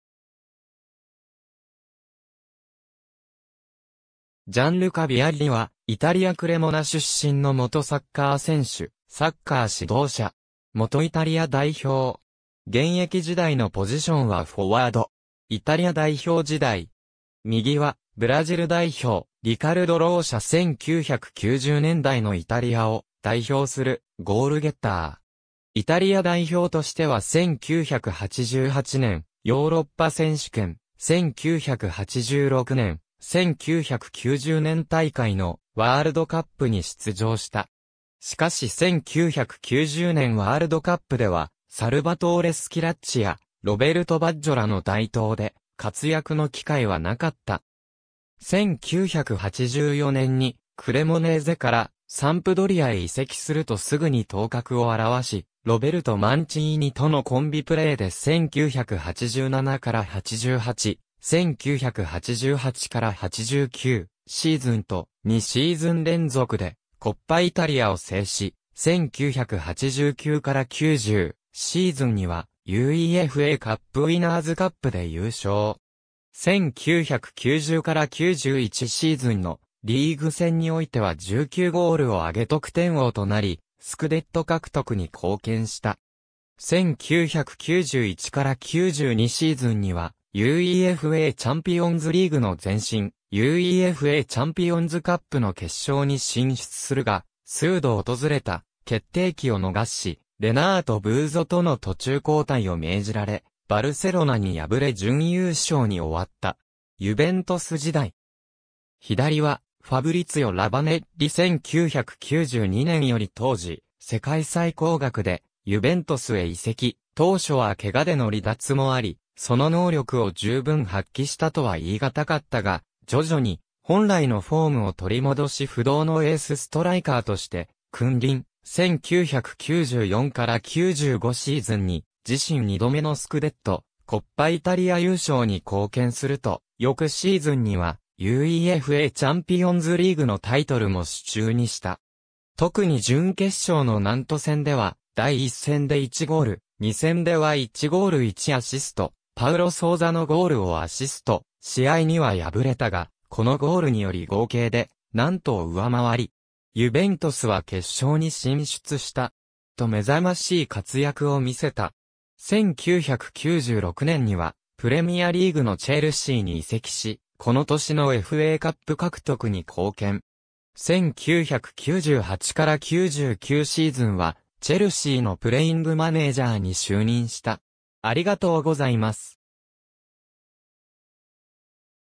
ジャンルカ・ヴィアッリ
ジャンルカ・ヴィアッリ（Gianluca Vialli, 1964年7月9日 - ）は、イタリア・クレモナ出身の元サッカー選手、サッカー指導者。
元イタリア代表。
現役時代のポジションはフォワード。
イタリア代表時代。
右はブラジル代表リカルド・ローシャ （1989年） 1990年代のイタリアを代表するゴールゲッター。
イタリア代表としては1988年ヨーロッパ選手権、1986年、1990年大会のワールドカップに出場した。
しかし1990年ワールドカップではサルヴァトーレ・スキラッチやロベルト・バッジョらの台頭で活躍の機会はなかった。
1984年にクレモネーゼからサンプドリアへ移籍すると直ぐに頭角を現し、ロベルト・マンチーニとのコンビプレーで1987-88、1988-89シーズンと、2シーズン連続でコッパ・イタリアを制し、1989-90シーズンにはUEFAカップウィナーズカップで優勝。
1990-91シーズンのリーグ戦においては19ゴールを挙げ得点王となり、スクデット獲得に貢献した。
1991-92シーズンにはUEFAチャンピオンズリーグの前身、UEFAチャンピオンズカップの決勝に進出するが、数度訪れた決定機を逃し、レナート・ブーゾとの途中交代を命じられ、バルセロナに敗れ準優勝に終わった。
ユヴェントス時代。
左はファブリツィオ・ラヴァネッリ（1995年） 1992年より当時世界最高額でユヴェントスへ移籍、当初は怪我での離脱もあり、その能力を十分発揮したとは言い難かったが、徐々に本来のフォームを取り戻し不動のエースストライカーとして君臨、1994-95シーズンに自身2度目のスクデット、コッパイタリア優勝に貢献すると、翌シーズンにはUEFAチャンピオンズリーグのタイトルも手中にした。
特に準決勝のナント戦では、第1戦で1ゴール、2戦では1ゴール1アシスト(パウロ・ソウザのゴールをアシスト、試合には敗れたが、このゴールにより合計でナントを上回り。
ユベントスは決勝に進出した。
)と目覚ましい活躍を見せた。
1996年にはプレミアリーグのチェルシーに移籍し、この年のFAカップ獲得に貢献。
1998-99シーズンはチェルシーのプレイングマネージャーに就任した。